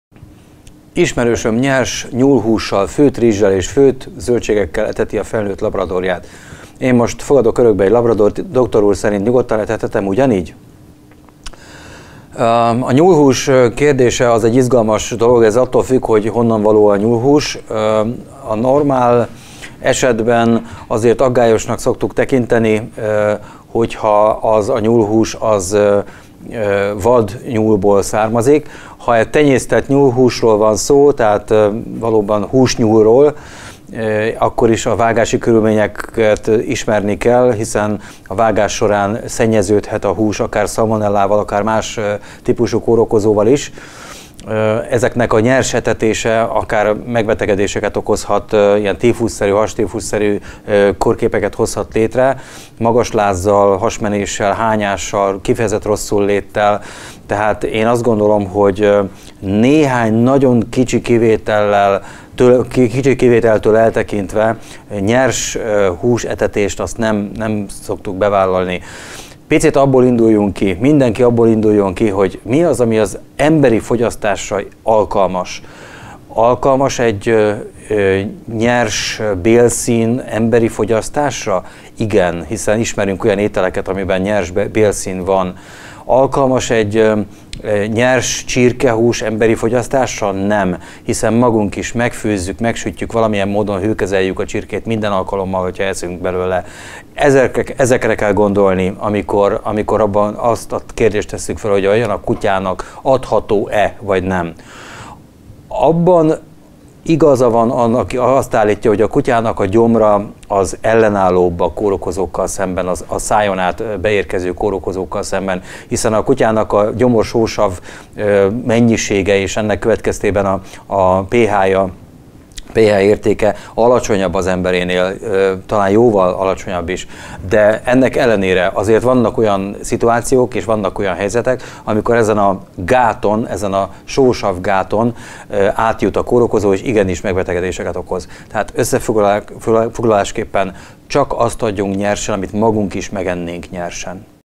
Nyúlhúst az ebnek! Vagy mégsem?
Adhatunk-e a kutyának nyers nyúlhúst? Az állatorvos szerint: a nyúlhús megfelelhet, ha jól ellenőrizhető helyről származik. Különösképpen óvatosnak kell lennünk a vadnyúl húsával. A szennyezett nyúlhús akár tífusszerű, súlyos megbetegedést is okozhat a kutyánál.